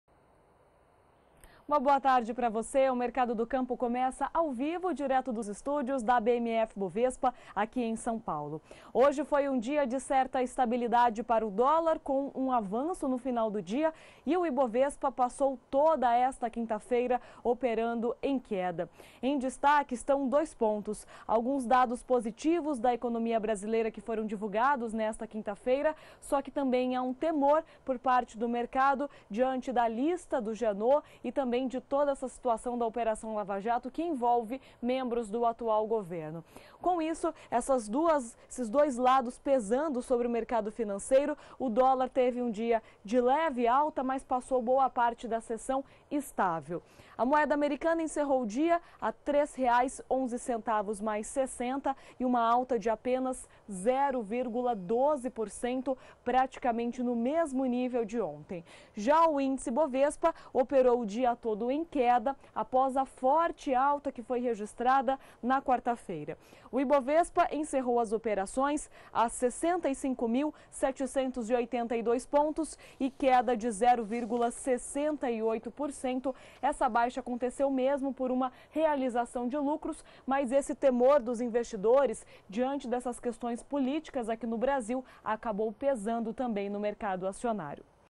DÓLAR + IBOVESPA - 16/03/2017
O dólar comercial opera próximo da estabilidade, diante da cautela dos investidores com a cena politica domestica de um lado e dados positivos do noticiário econômico do outro.